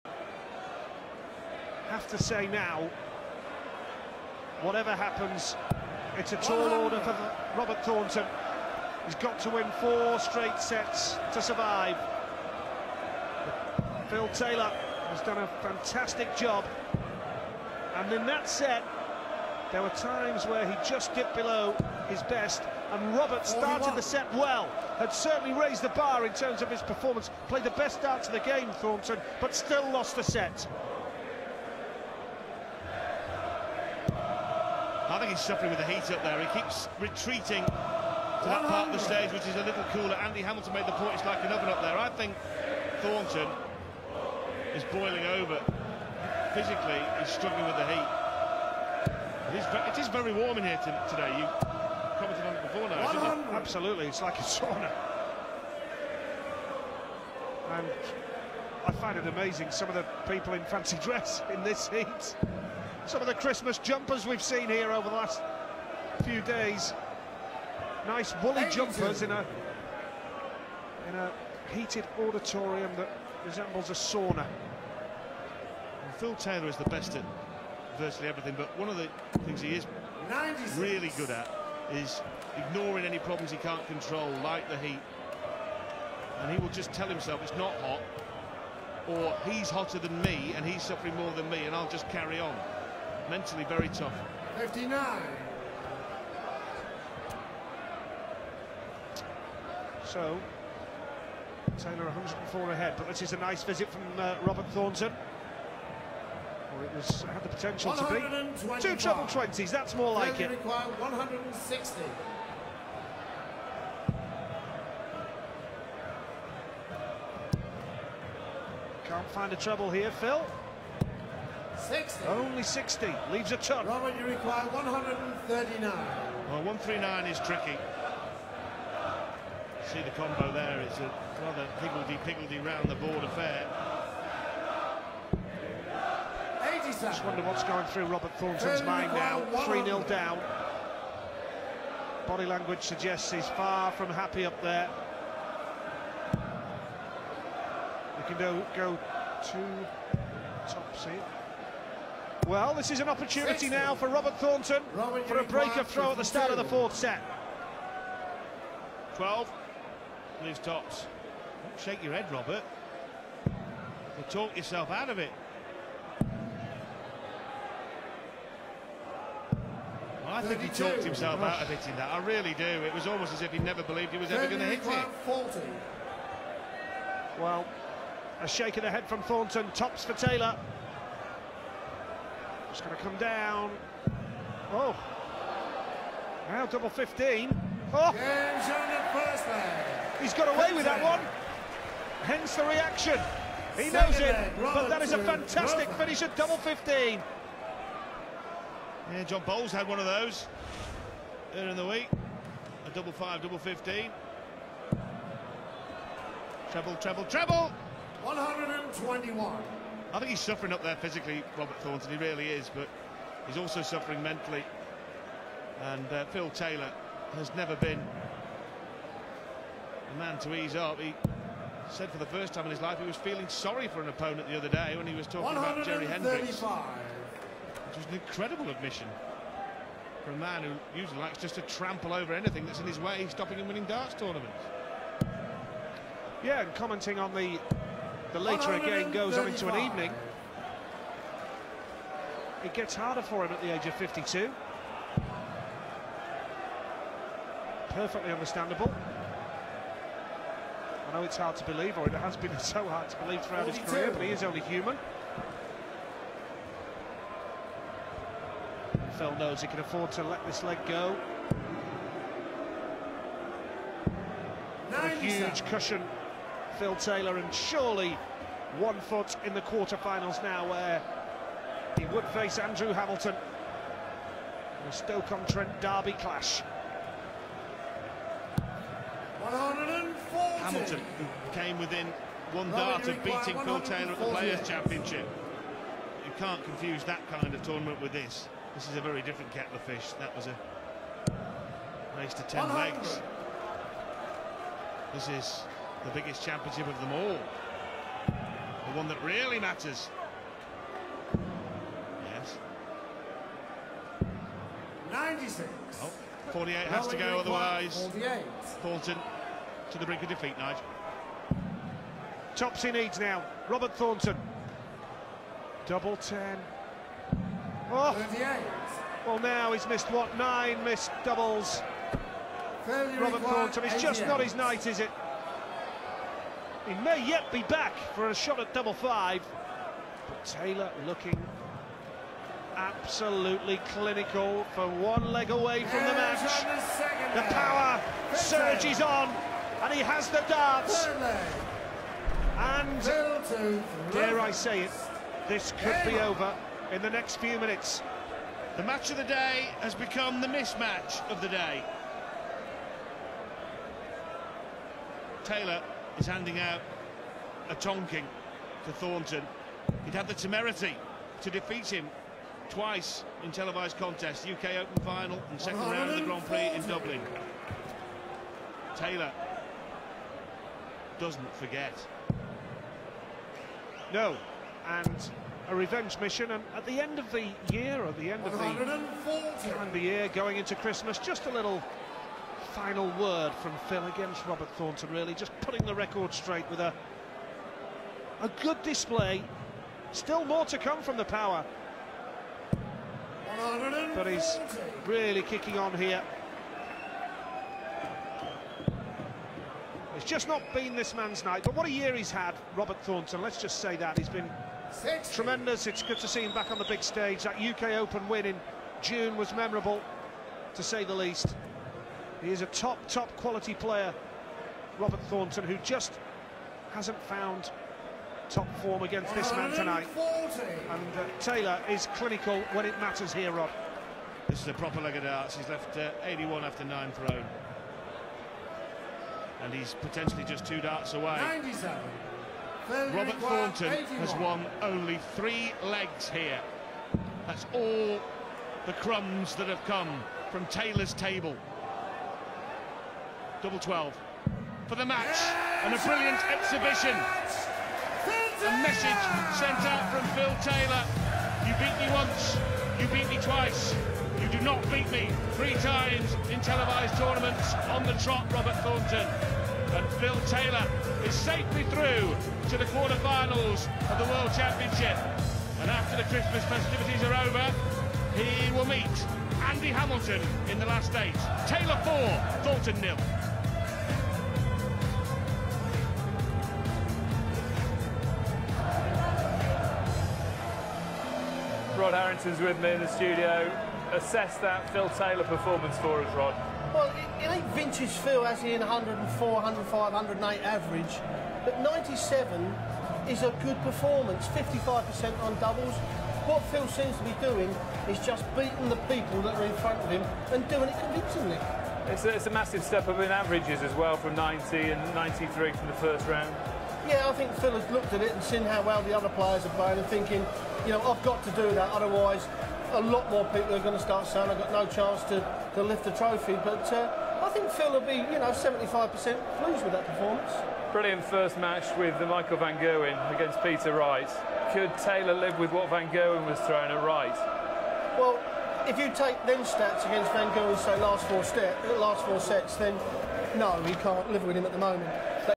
Taylor v Thornton | 4/4 | Third Round | World Darts Championship 2013
Phil Taylor v Robert Thornton | 4/4 | Third Round | PDC Ladbrokes World Darts Championship 2013